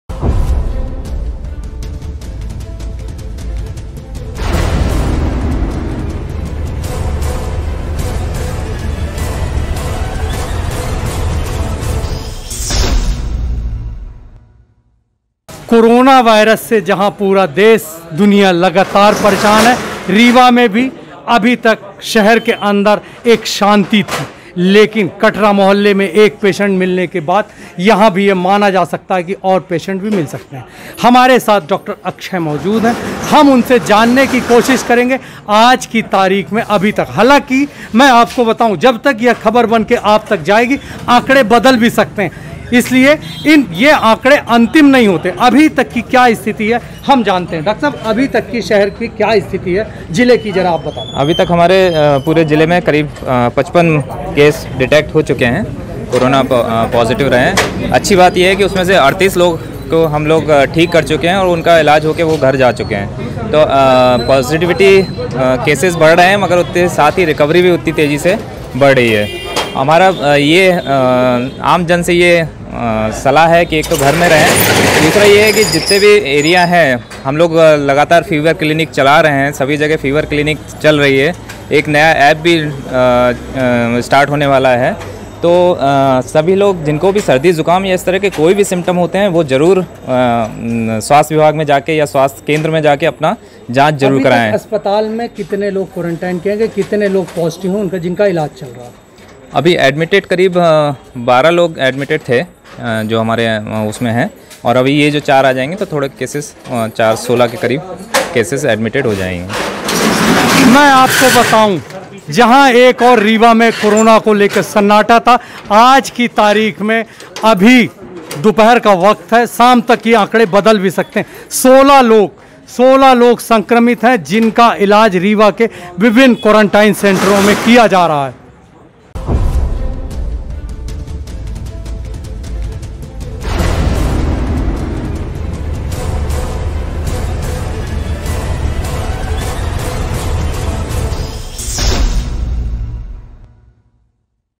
REWA में 16 एक्टिव कोरोना संक्रमित मरीज ||NNT
REWA में 16 एक्टिव कोरोना संक्रमित मरीज